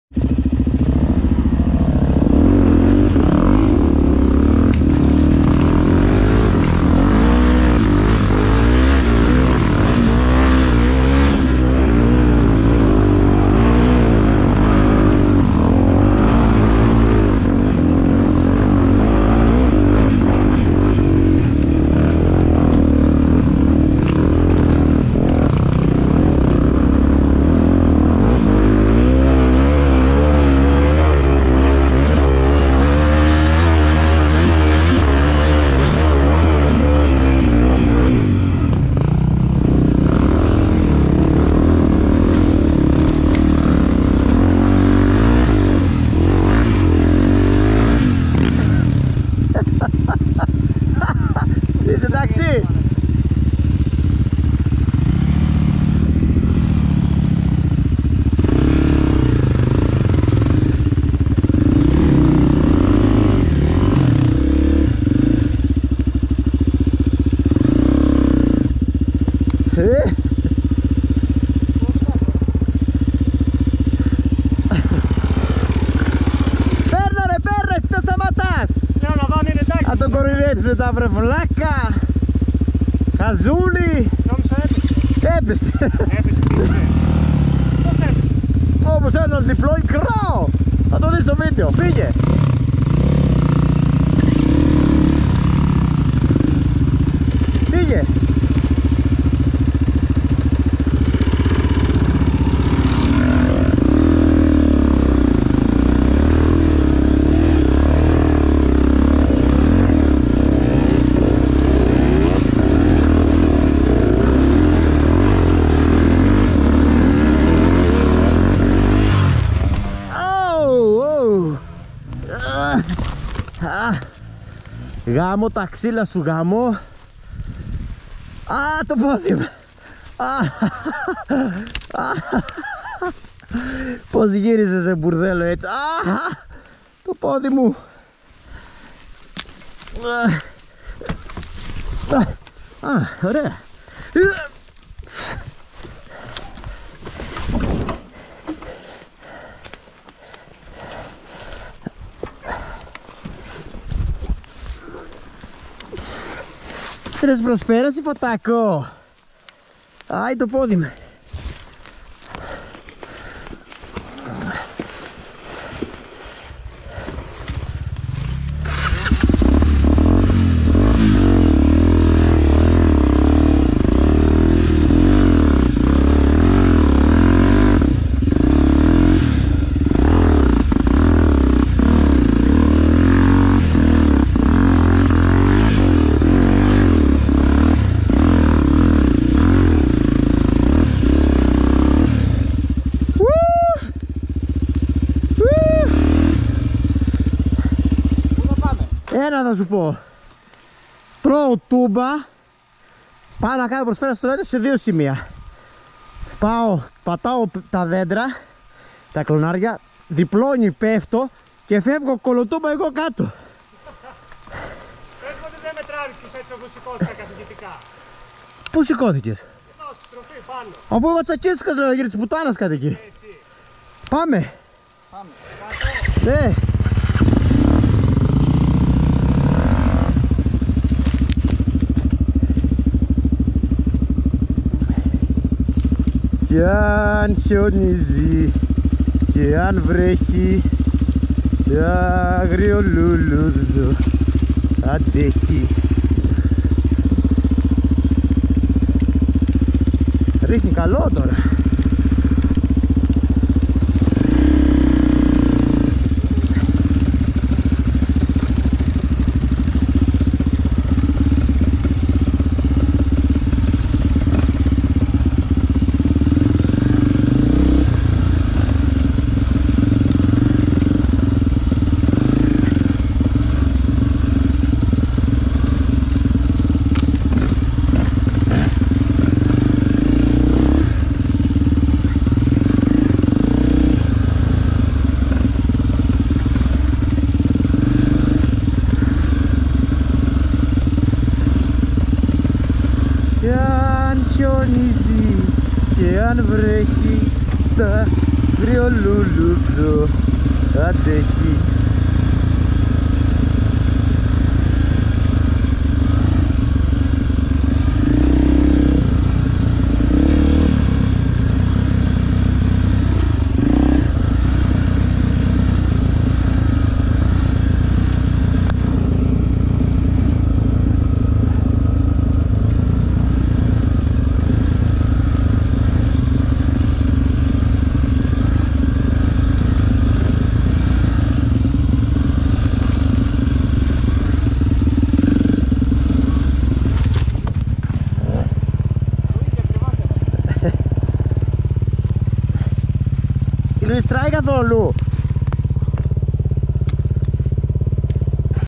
KARPENISSI ENDURO DOKIMI NEOY MONOPATIOY
άνοιγμα νέου μονοπατιού για το Enduro day Karpenisi v2 στις 04/06/2023 στο Καρπενήσι!